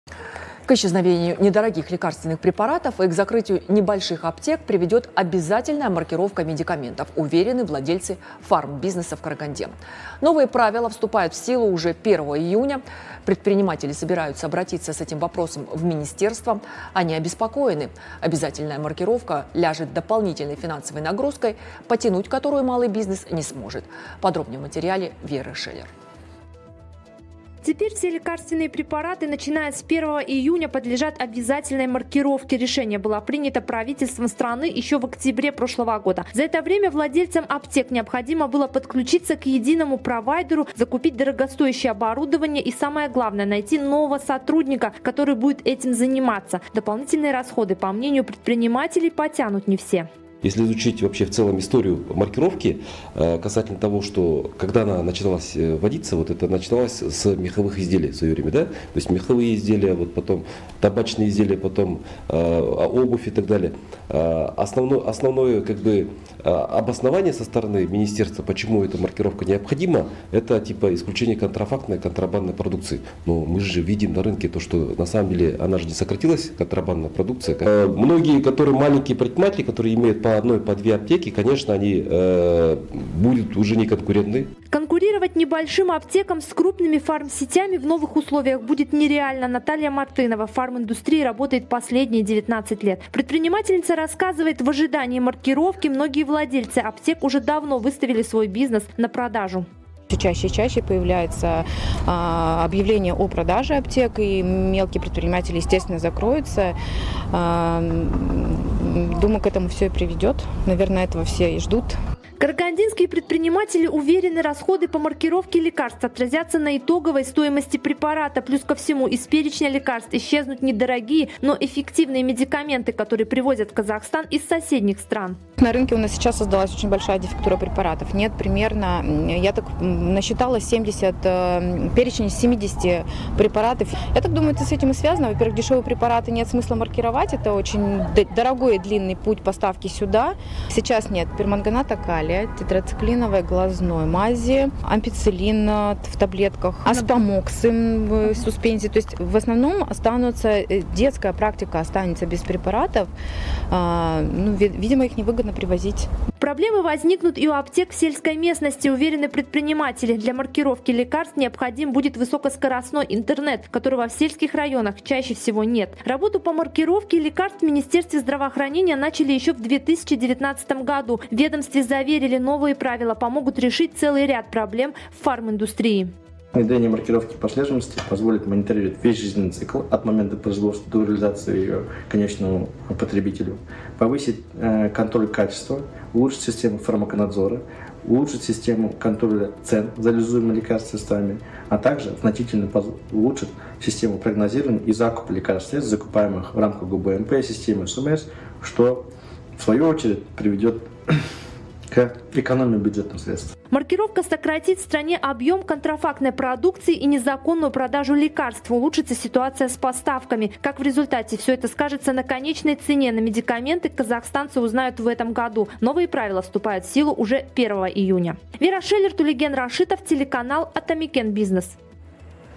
Обязательная маркировка лекарств приведет к закрытию небольших аптек
К исчезновению не дорогих лекарственных препаратов и к закрытию небольших аптек приведет обязательная маркировка медикаментов, уверены владельцы карагандинских аптек. Новые правила, которых так боятся казахстанские фармкомпании, вступают в силу уже 1 июня. Карагандинские предприниматели собираются обращаться с этим вопросом в министерство. Они уверены: обязательная маркировка ляжет дополнительной финансовой нагрузкой на плечи предпринимателей, потянуть которую малый бизнес не сможет. Подробнее смотрите в материале нашего регионального корреспондента Веры Шеллер.